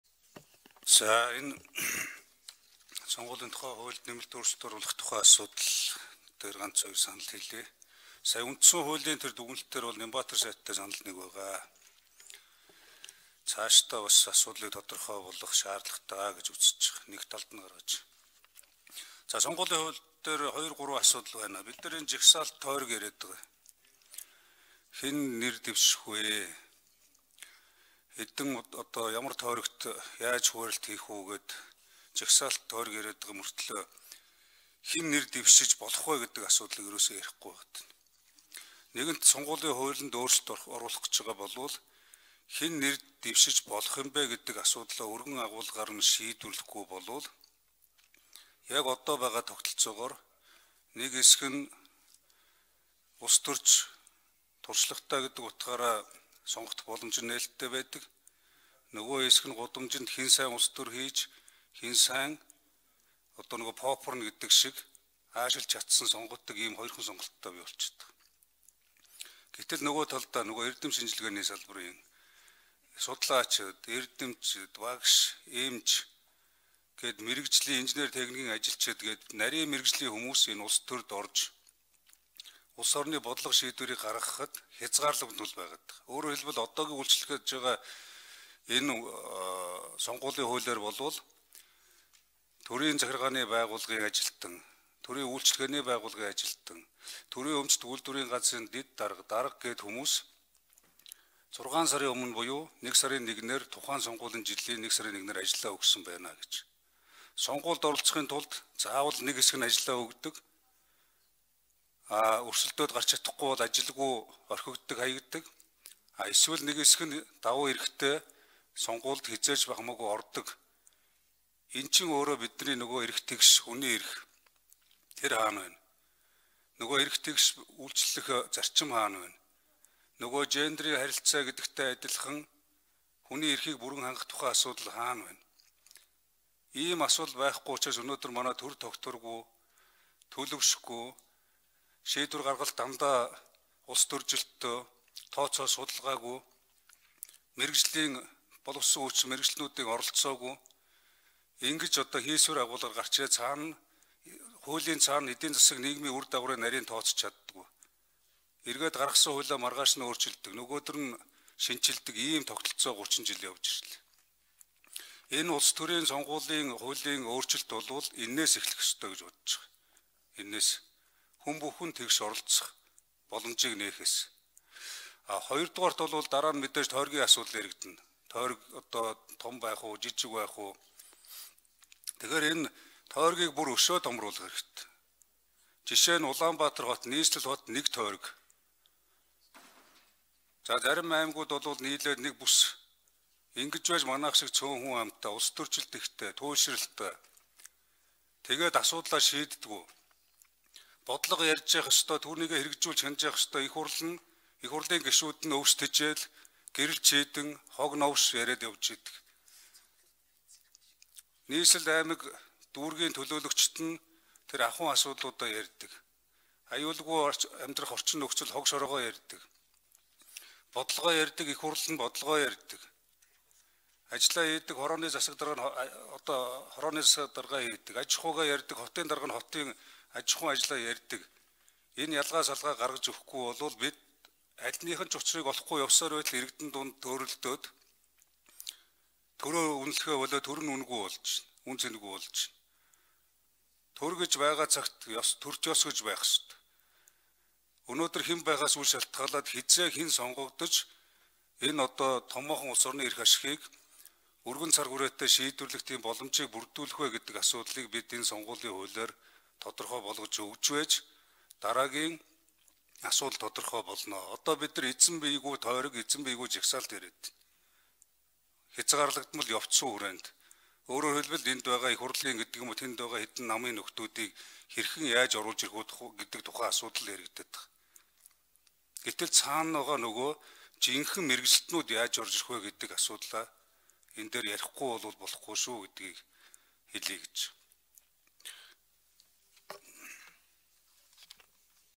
С.Амарсайхан: Сонгуулийн тухай хуулийн нэмэлт, өөрчлөлтөд хэн нэр дэвшиж болох вэ гэдгийг тодорхой
С.Амарсайхан: Сонгуулийн тухай хуулийн нэмэлт, өөрчлөлтөд хэн нэр дэвшиж болох вэ гэдгийг тодорхой болгох шаадлагатай